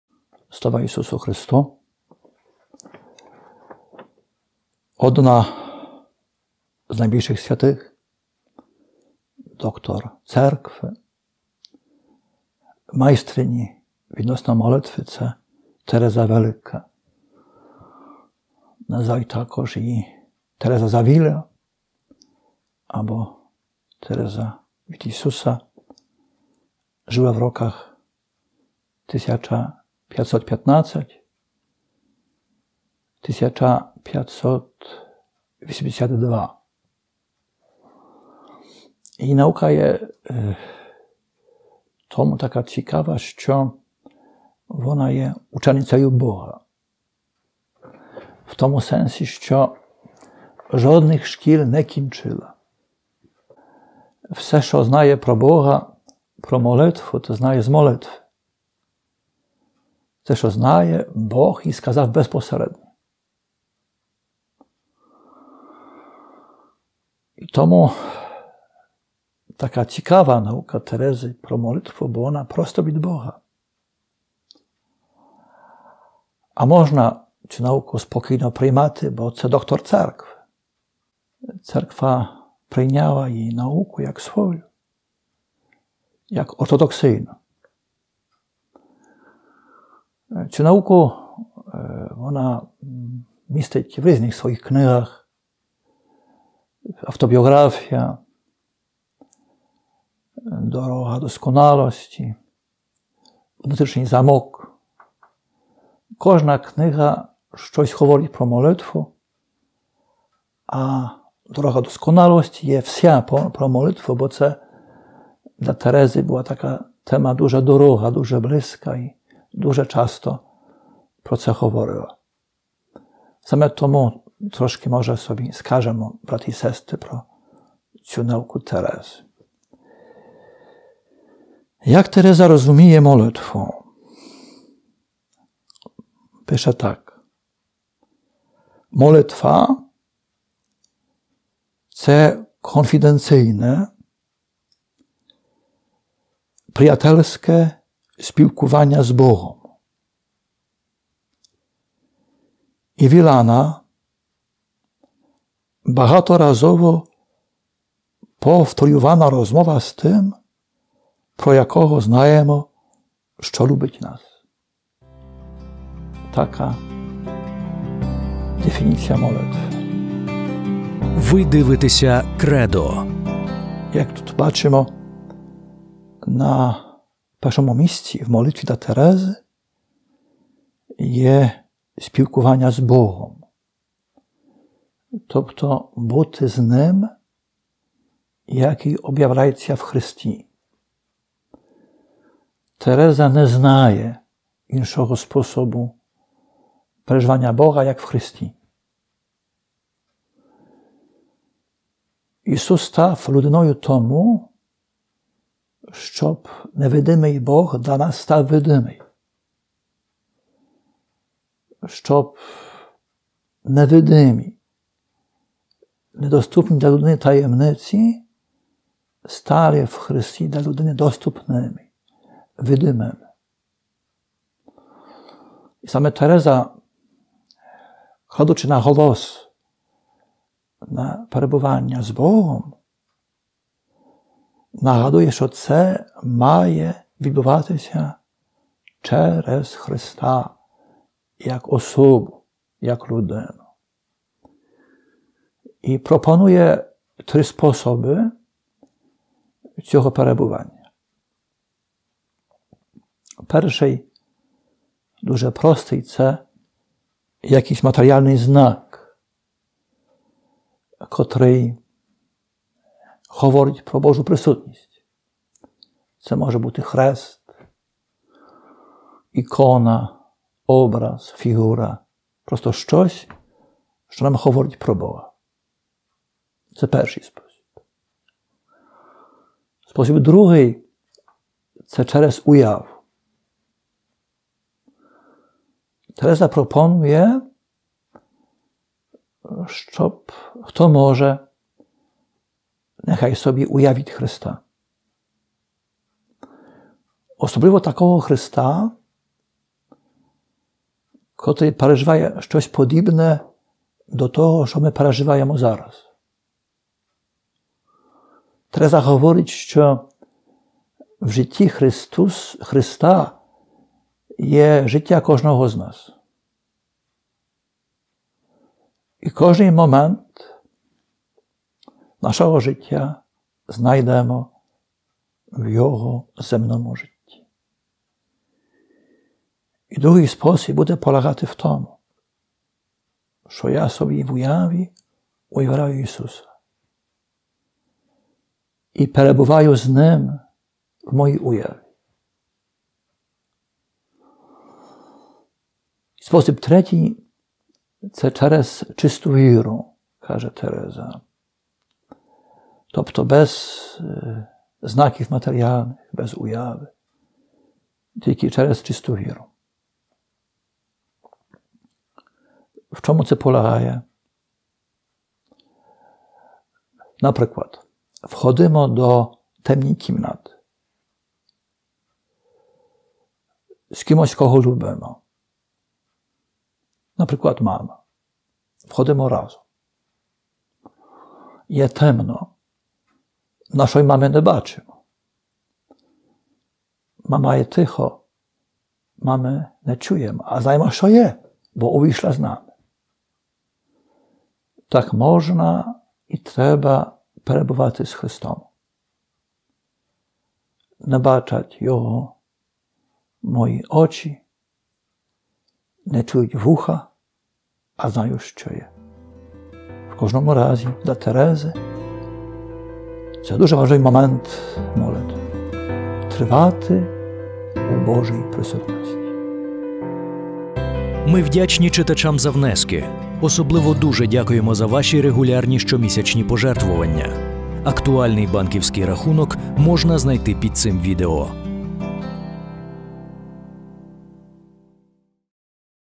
Три способи перебування з Богом від святої Терези Авільської
Отець Ришард Федерчик, доктор теології, духівник Кам’янець-Подільської дієцезії, говорить про молитву. Сьогодні священик розповідає про те, як молитву бачила св. Тереза Авільська.
Вчення святої Терези Авільської про молитву можна вважати ортодоксійним, адже вона є Доктором Церкви. Вона — учениця Бога. Тереза не закінчувала жодних шкіл і все, що вона знає про молитву — знає з молитви. Тому таке цікаве вчення святої Терези про молитву, бо походить від Бога.

Молитва — це конфіденційне приятельське спілкування з Богом і багаторазово повторювана розмова з Тим, Хто нас любить, — каже св. Тереза.

1. Матеріальний знак, який говорить про Божу присутність: хрест, ікона, образ, статуя — щось, що нам говорить про Бога.

2. Уява. Свята Тереза пропонує тим, хто може, уявити собі Христа, Який переживає щось подібне до того, що ми переживаємо зараз. Адже в житті Христа є життя кожного з нас і кожен момент із нашого життя ми знайдемо в Його земному житті.

3. Чиста віра. Без жодних матеріальних знаків, без жодної уяви. Наприклад, якщо ми перебуваємо в темній кімнаті з Кимось, Кого любимо, знаємо, що Він там є, навіть якщо Його не бачимо і не чуємо.

• для збору мобільних переказів: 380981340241